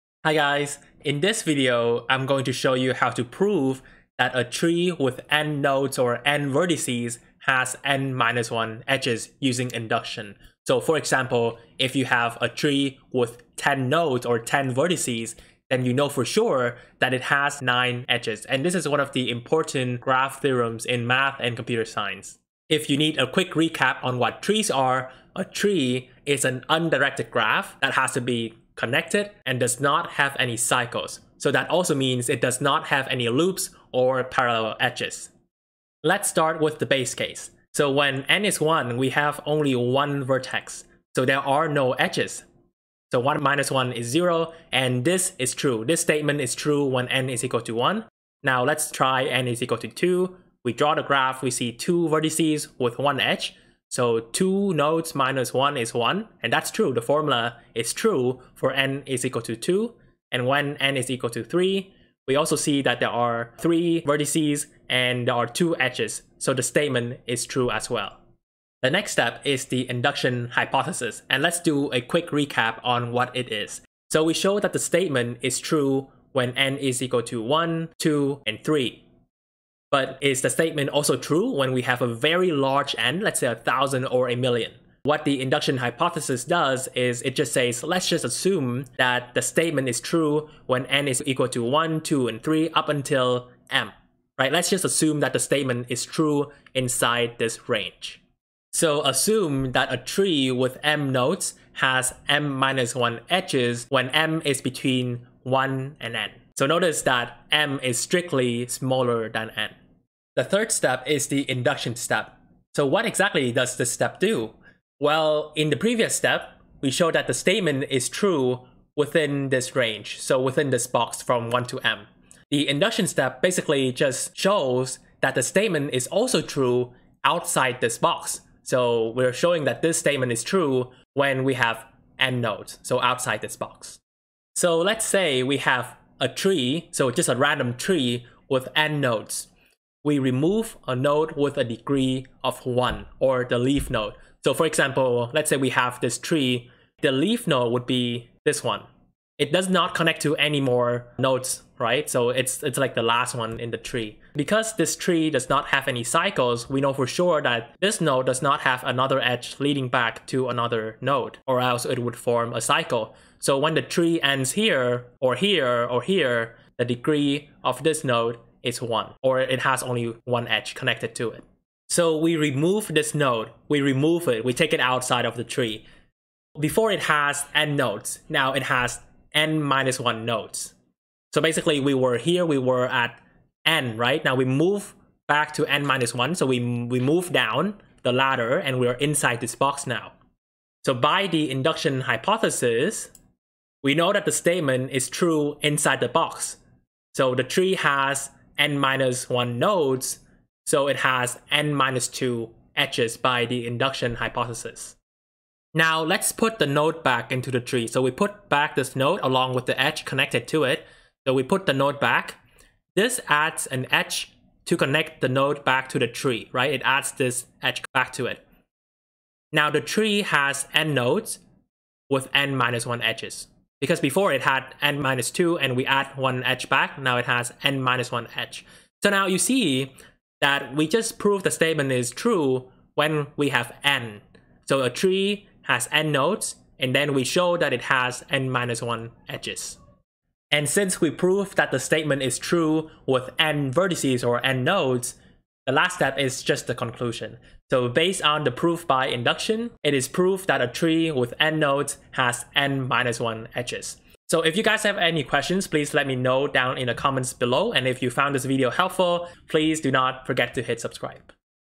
Prove that a tree with n vertices has n-1 edges
In this video, I will show you how to prove that a tree with n vertices or nodes has n-1 edges using proof by induction. For example, if you are given a tree with 10 vertices or nodes, then you know for sure that there are 9 edges. So, what exactly is a tree? A tree is an undirected graph that is a connected graph and has no cycles, parallel edges, or loops. I will prove this statement using mathematical statement. The induction proof that a tree of n nodes has n-1 edges is a basic theorem in graph theory which is important in both discrete math and computer science classes. If you are given a graph of n vertices with no cycles, loops, or parallel edges, and you find out that the graph has exactly n-1 edges, then you know that it is a tree.